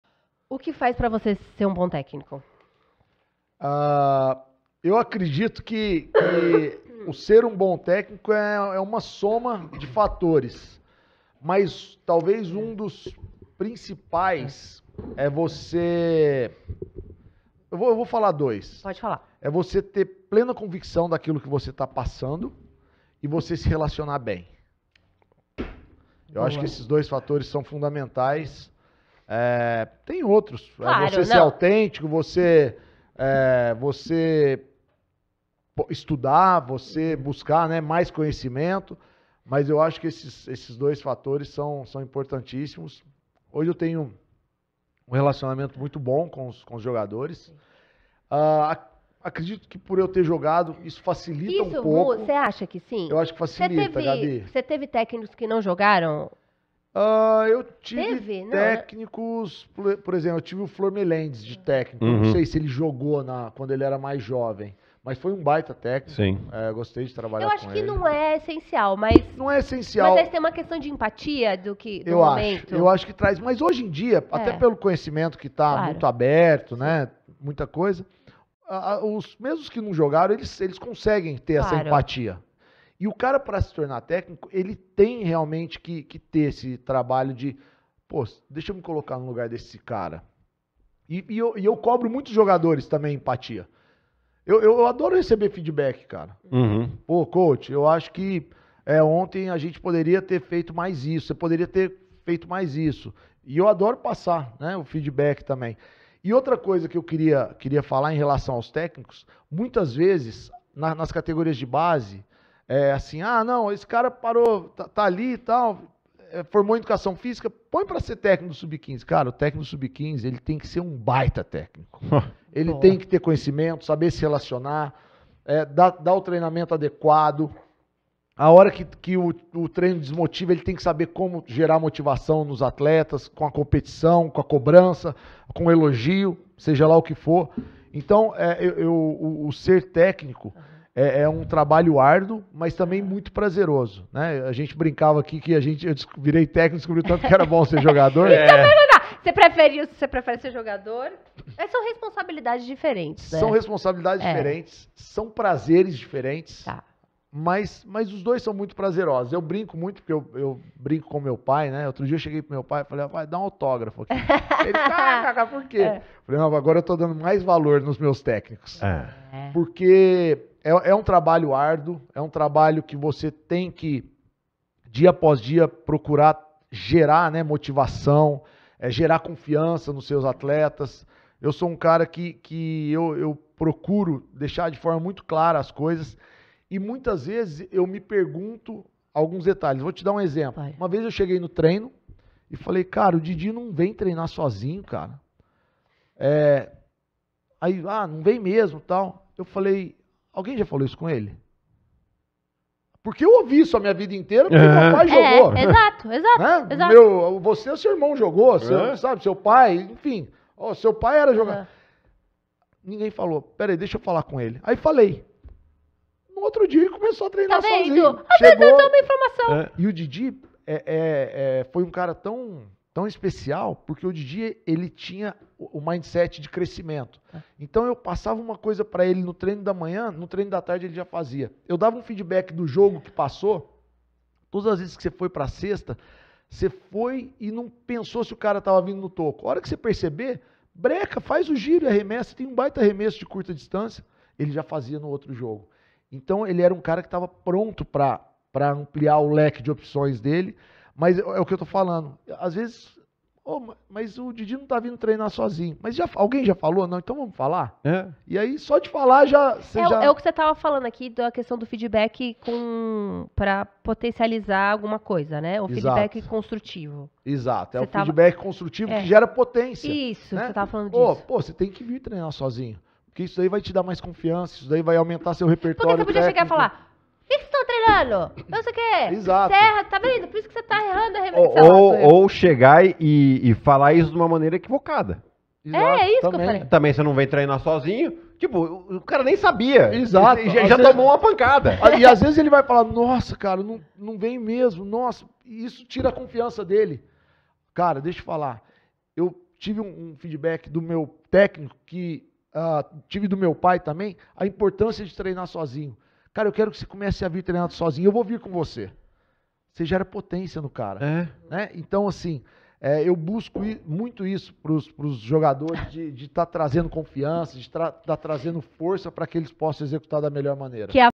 O MEU RELACIONAMENTO COM OS ATLETAS - HELINHO
Como ser um bom técnico no s dias atuais? Helinho fez duas ponderações.